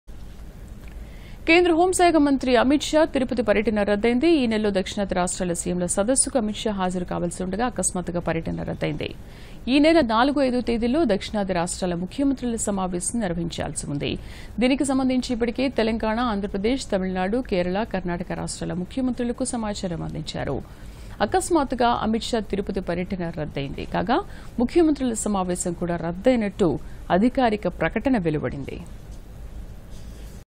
Union Home Minister Amit Shah's visit to Tirupati has been canceled.
కేంద్ర హోం శాఖ మంత్రి అమిత్‌ షా తిరుపతి పర్యటన రద్దయ్యింది. ఈనెలలో దక్షిణాది రాష్ట్రాల సీఎంల సదస్సుకు అమిత్ షా హాజరుకావాల్సి ఉండగా అకస్మాత్తుగా పర్యటన రద్దయ్యింది. ఈనెల 4, 5 తేదీల్లో దక్షిణాది రాష్ట్రాల ముఖ్యమంత్రుల సమావేశం నిర్వహించాల్సి ఉన్నది. దీనికి సంబంధించి ఇప్పటికే తెలంగాణ, ఆంధ్రప్రదేశ్‌, తమినాడు, కేరళ, కర్ణాటక రాష్ట్రాల ముఖ్యమంత్రులకు సమాచారం అందించారు. అకస్మాత్తుగా అమిత్‌షా తిరుపతి పర్యటన రద్దు అయ్యింది. కాగా, ముఖ్యమంత్రుల సమావేశం కూడా రద్దయినట్టు అధికారిక ప్రకటన వెలువడింది.